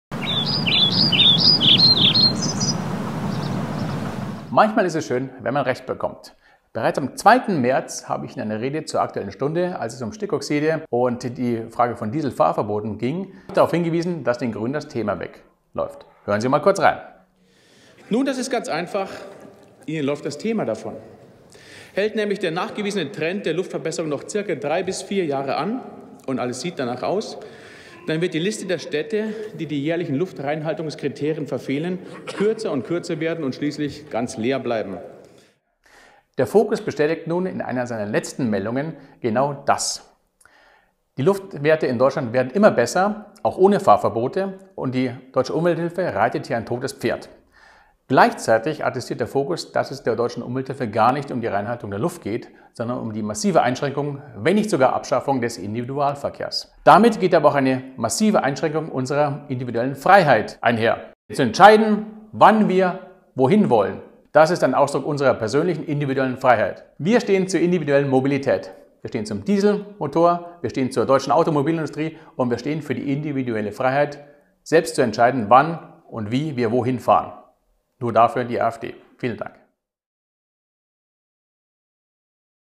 Zwischenmeldung
Auch in der Sommerpause dreht die Welt sich weiter und wie angekündigt, berichte ich in der sitzungsfreien Zeit aus meinem Wahlkreis.
Heute: Die Ziele der "Deutschen Umwelthilfe"